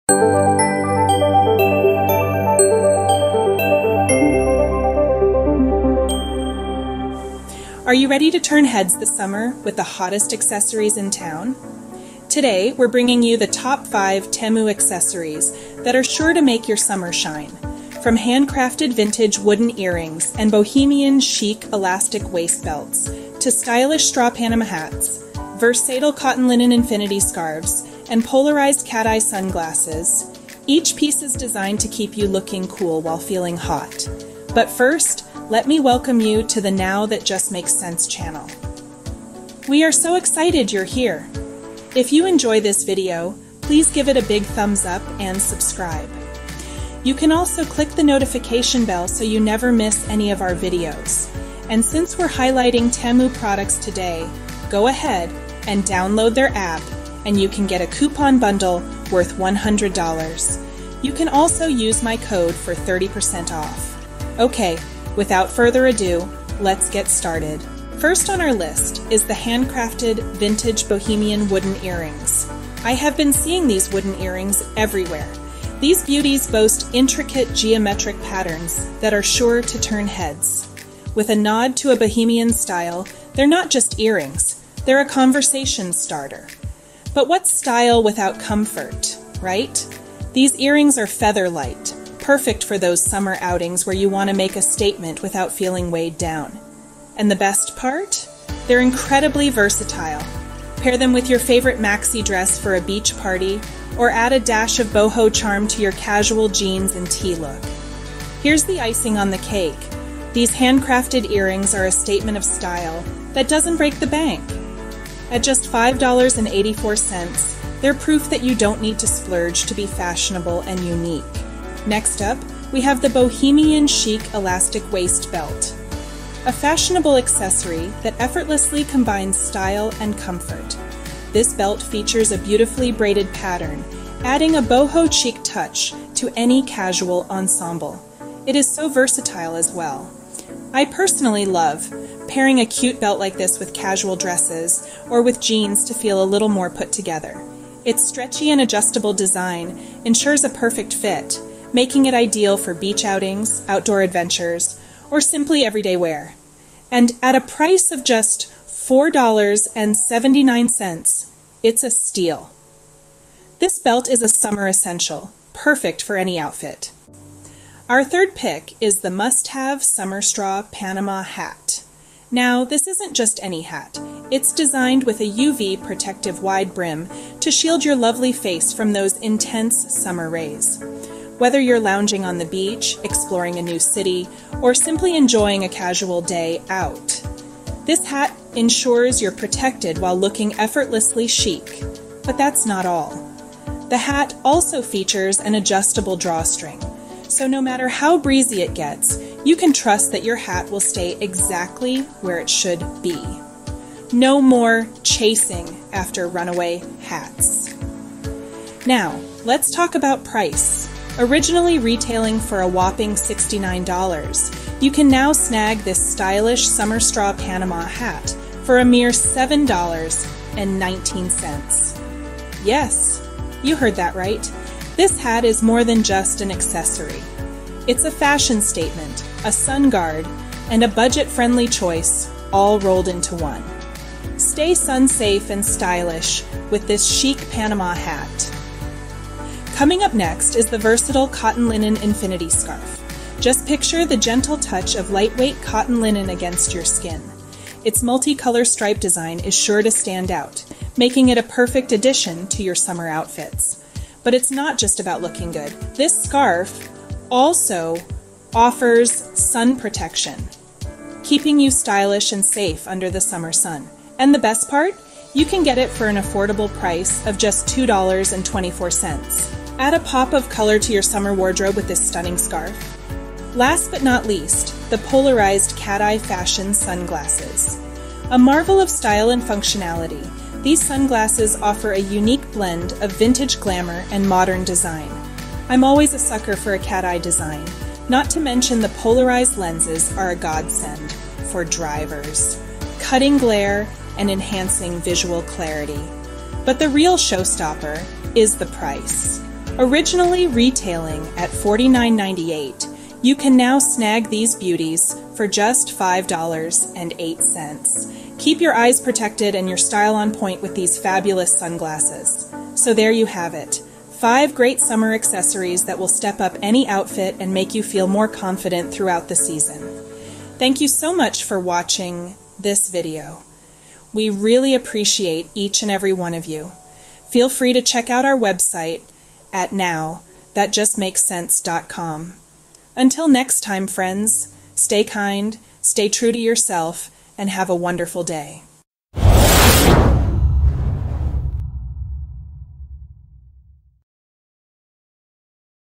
Top 5 Temu Summer Accessories You Need Now!
👑Accessories are the key to elevating your style.✨ All the products are my top picks from @shoptemu !
】to download the Temu app and search my code 【ach344060】to get an exclusive discount.

Hey there, fashionistas! 🌞 Ready to elevate your summer style? In this video, we’re diving into the Top 5 Must-Have Summer Accessories from TEMU that you need in your wardrobe this season. From chic sunglasses to a stylish beach hat, we've got you covered with the hottest trends of Summer 2024.

10 Pairs of Handcrafted Vintage Bohemian Wooden Earrings - Intricate Geometric Ethnic Style Dangle for Women - Lightweight, Statement Jewelry Set, Ideal Fashion Accessory or Gift - $5.84

Bohemian Chic Elastic Waist Belt - Fashionable Braided Style for Casual Versatility - Stretchy & Adjustable, Perfect for Beach, Outdoor, and Daily Wear - $4.79

Chic Summer Straw Panama Hat for Women - UV Protective Wide Brim with Adjustable Drawstring for Beach Travels - $7.19 ($69.00 retail price)

1pc Cotton Linen Infinity Scarf, Multicolor Versatile Spring/Summer Stripe Design Neck Gaiter, Travel Sun Protection Basic Casual Head Wrap - $2.24 ($19.99 retail price)

Polarized Cat Eye Fashion Sunglasses For Women Drivers Brand Design Sun Shades For Driving Summer Beach Travel With Gifts Box Mother's Day/give Gifts - $5.08 ($49.98 retail price)

💬 Leave a comment to let us know your favorite summer accessory!